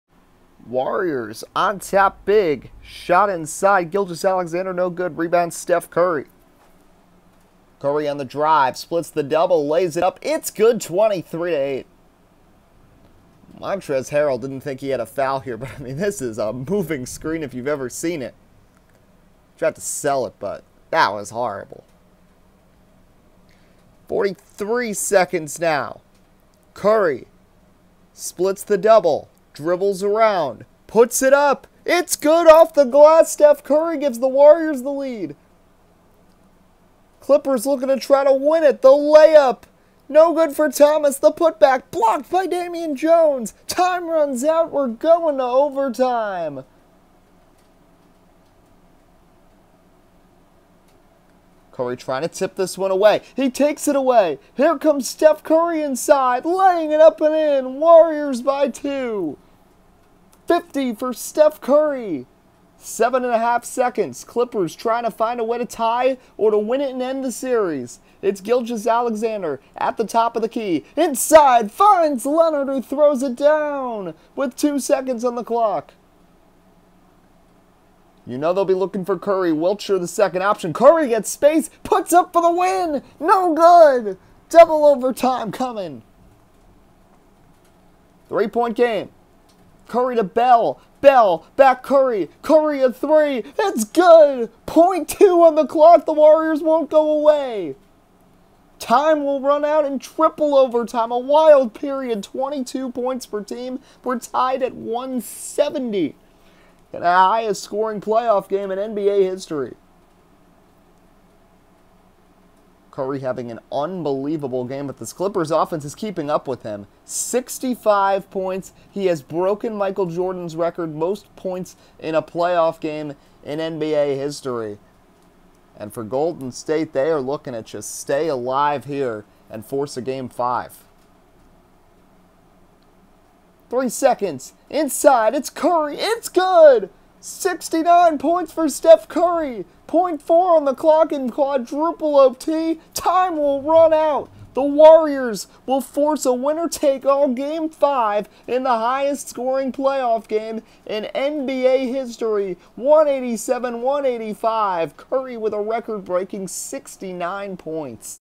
Record-Breaking Game!!! [NBA Playoffs Round 1 Game 4: Clippers at Warriors (Highlights) ]
A wild overtime affair between the Warriors and Clippers!